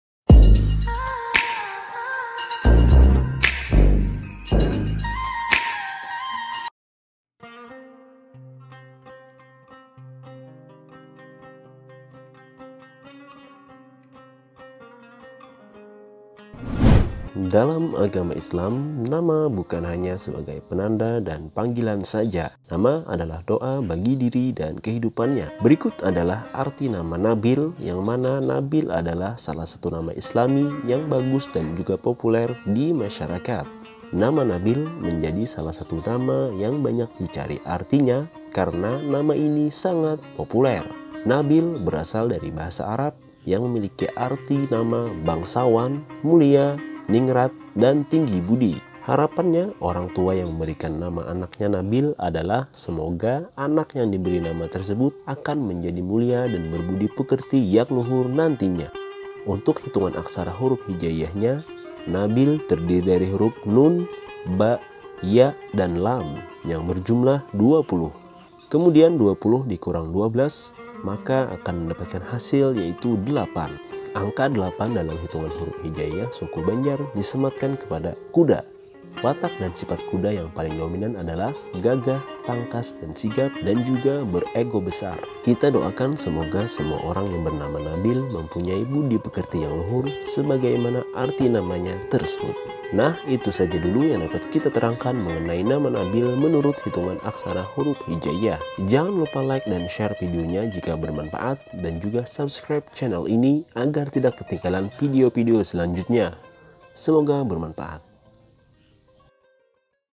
Arti Nama NABIL Dan Hitung Aksara Namanya
Berikut adalah video arti nama Nabil dan hitung aksara nama Nabil, yang mana Nabil adalah salah satu nama yang paling banyak dicari arti dan maknanya

nama Nabil mengandung makna yang bagus dan indah, ingin tau makna nama Nabil ?? tonton videonya sampai selesai ya guys